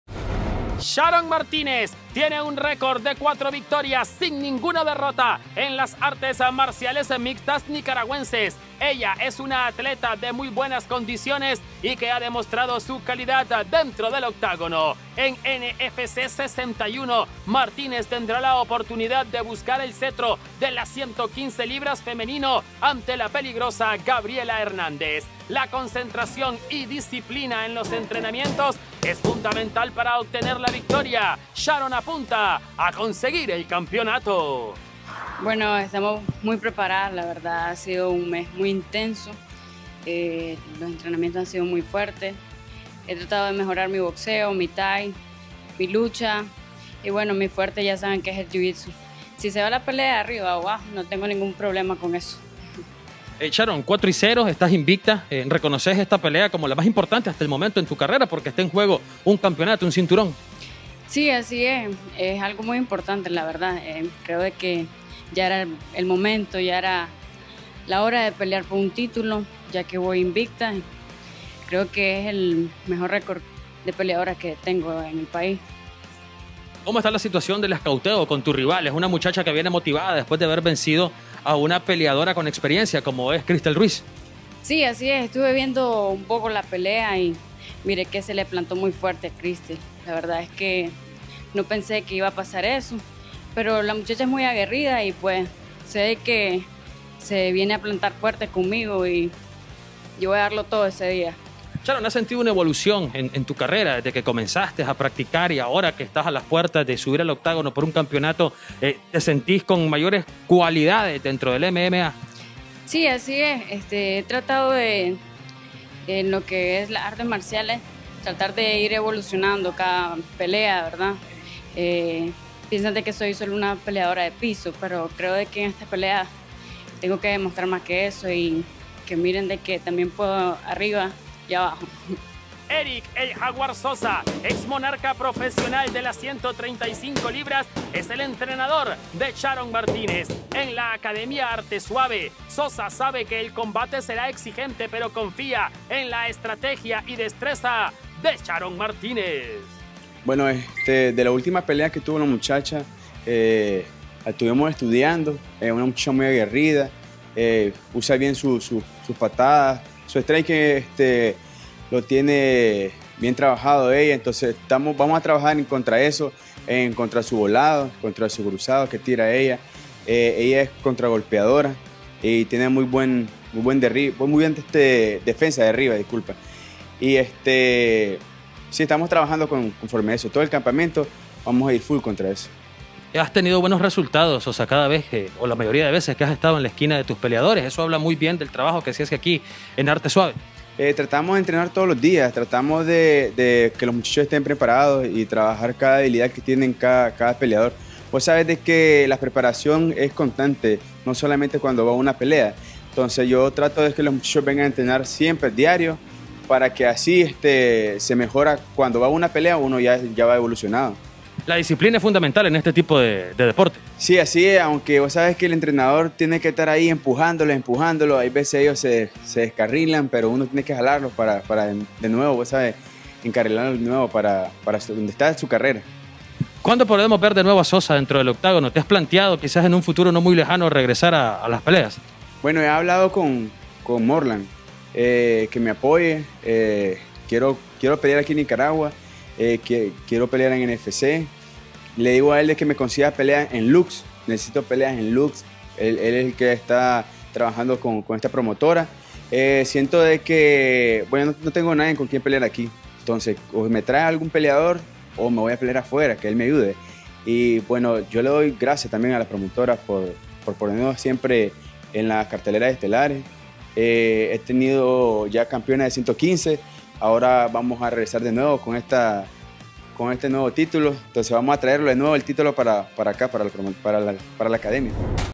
DEPORTIVOS || Sharon Martínez busca el título femenino en la NFC61
Sharon Martinez con un record de 4 victorias y 0 derrotas, busca el titulo de las 115 lbrs femenino.